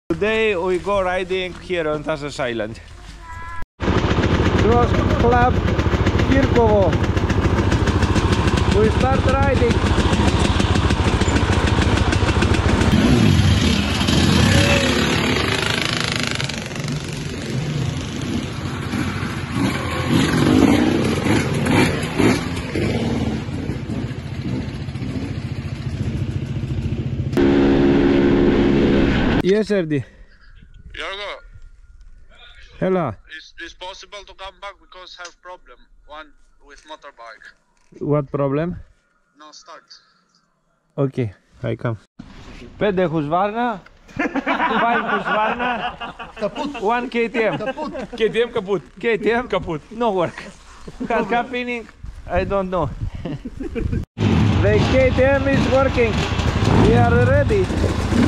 5 Husqvarna VS 1 KTM
Enduro riding with friends from Bulgaria Kirkovo